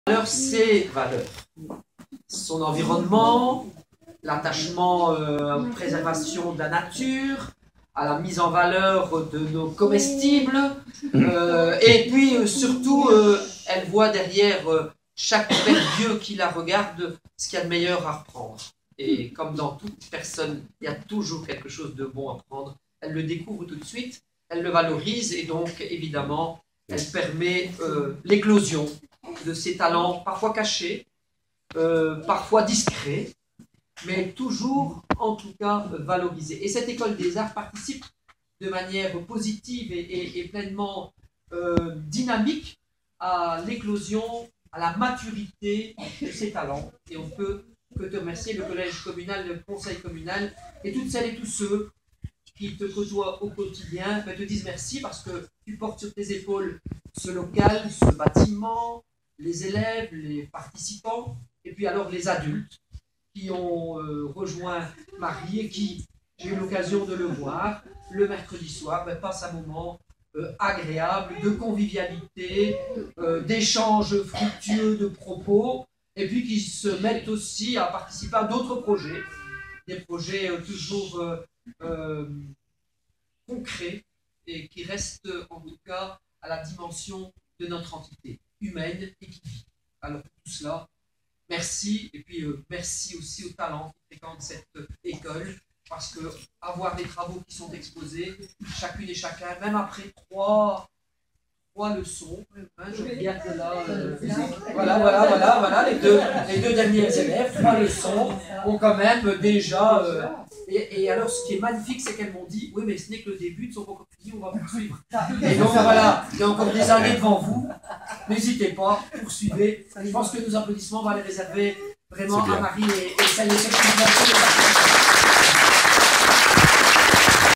Hommage du Bourgmestre à Marie Matterne la prof de l'école des arts de saint Léger